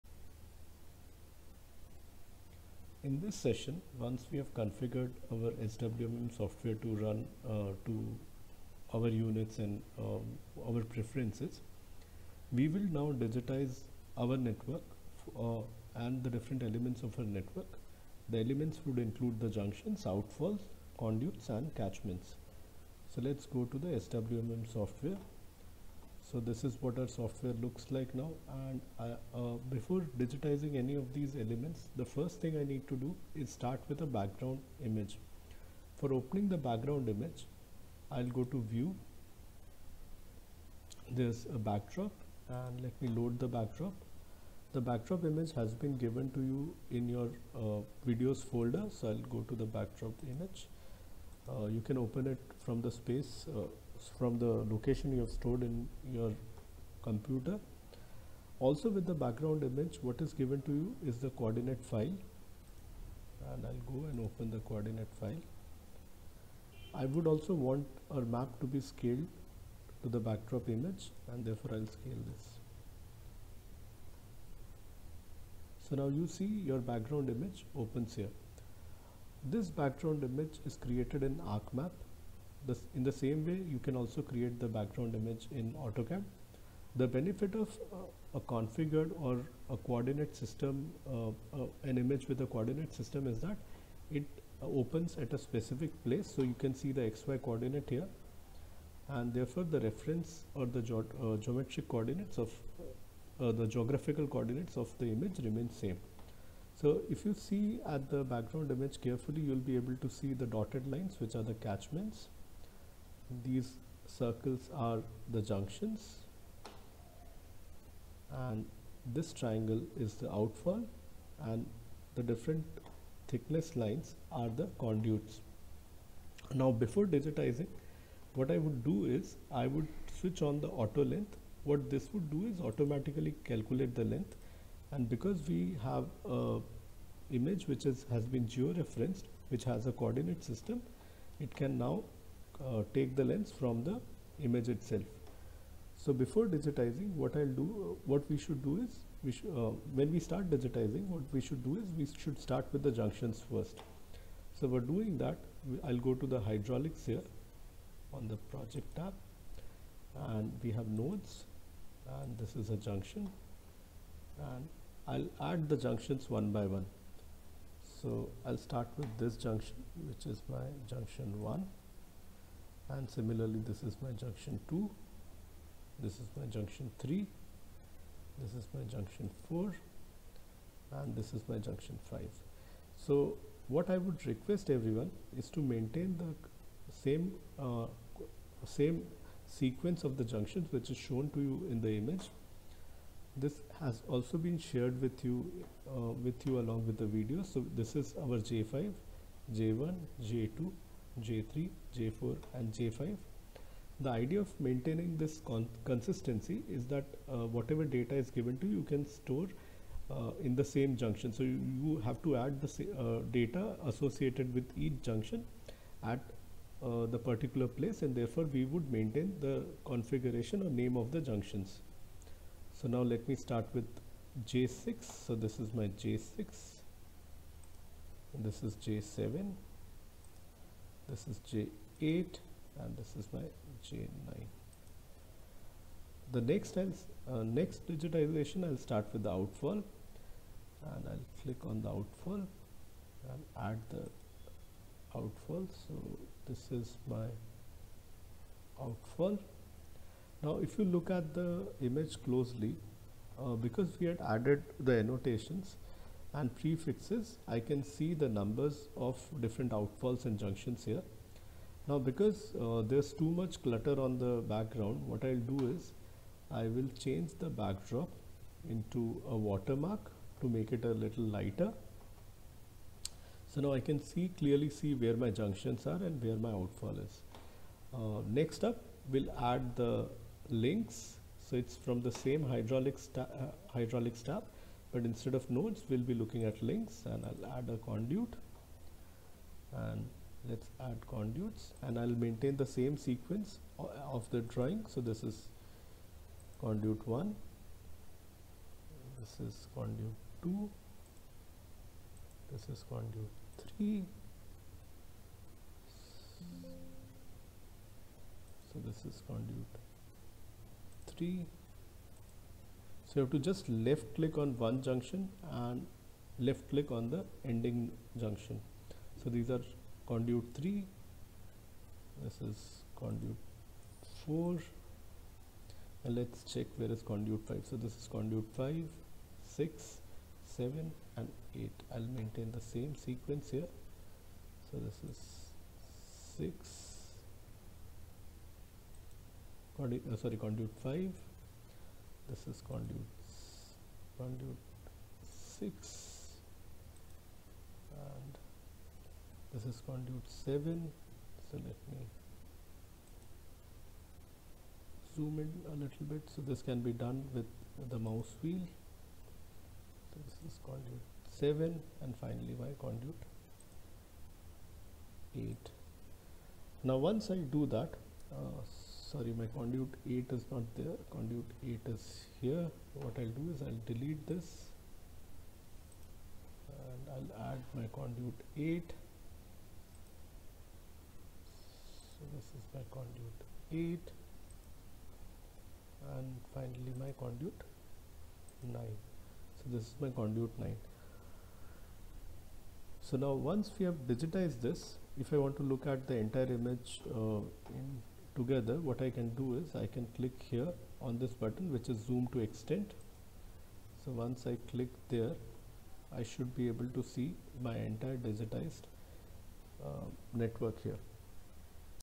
3. Digitizing the elements (Nodes and Links)
In this video, we start digitizing the nodes and links to see our model take shape.